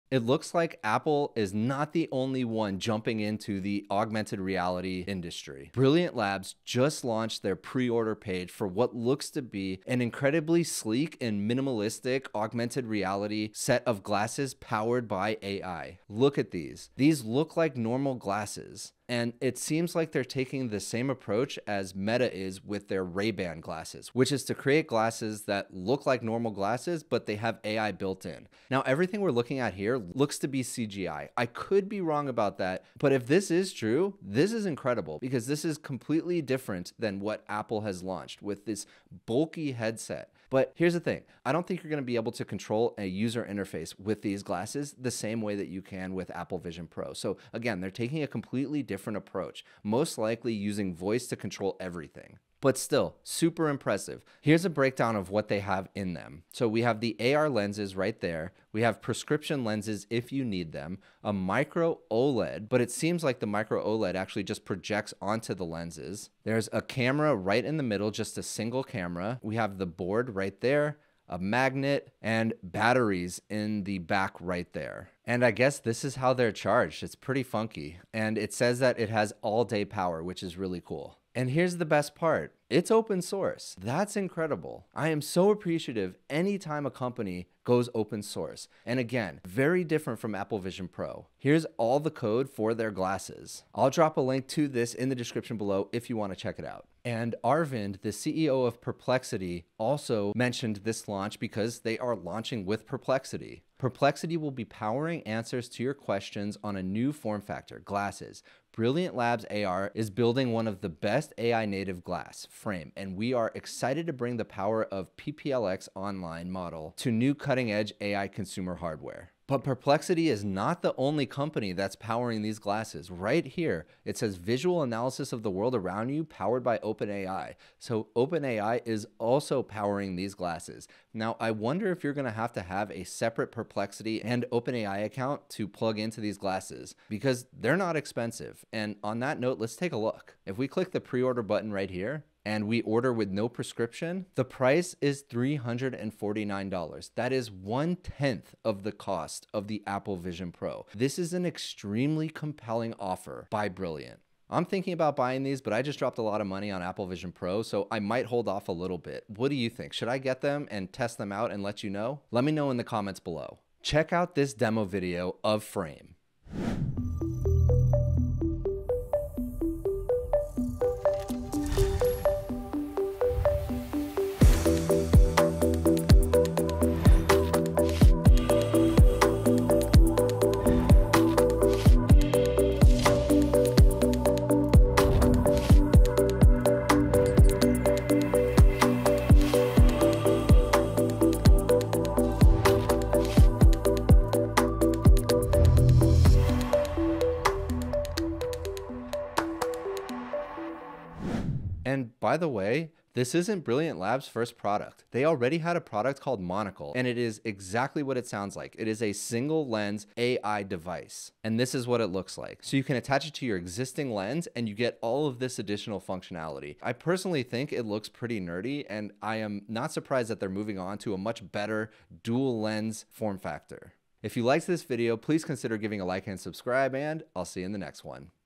100% Open-Source AI Glasses Only $349 (with OpenAI & Perplexity)
Frame is a new, open-source pair of AI glasses that looks promising. I'm thinking about buying a pair to try them out. I love all of these new AI devices and want to try them all, and I'm incredibly excited because this is open-source.